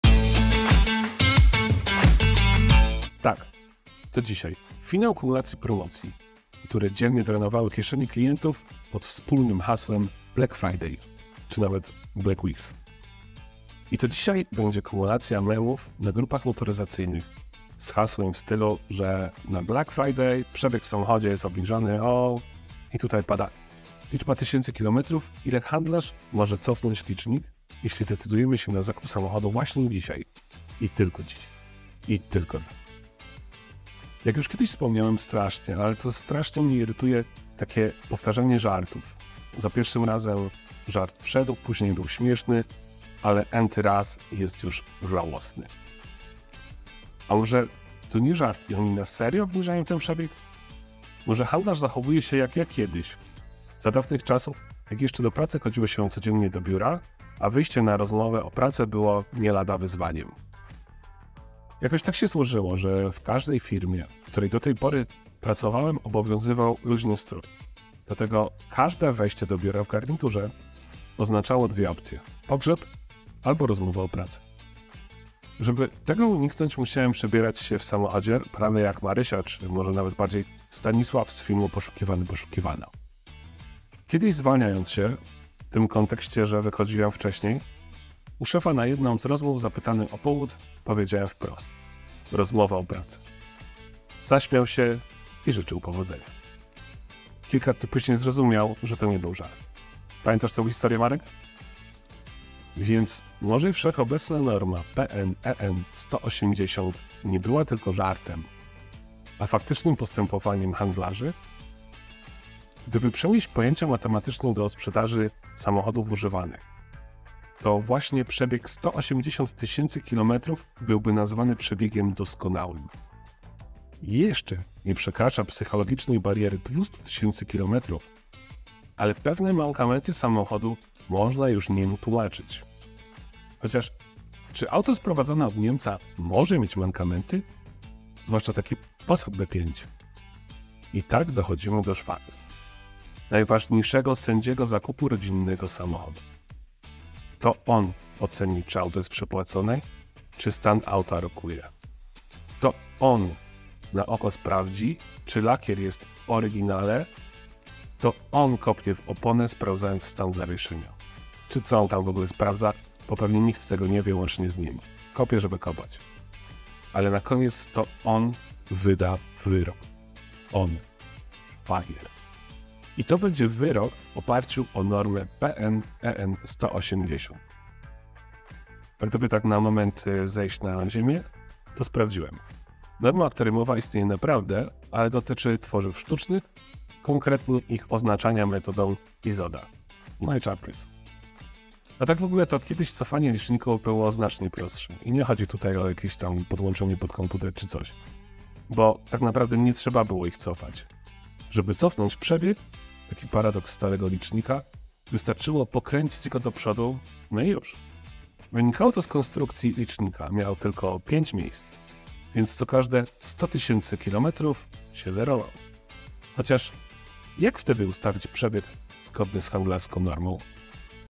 Przebieg z Black Friday | Felieton #005
Dowiedz się wiecej o starych samochodach i o tym, jak korzysta się z takiego samochodu na co codzień :)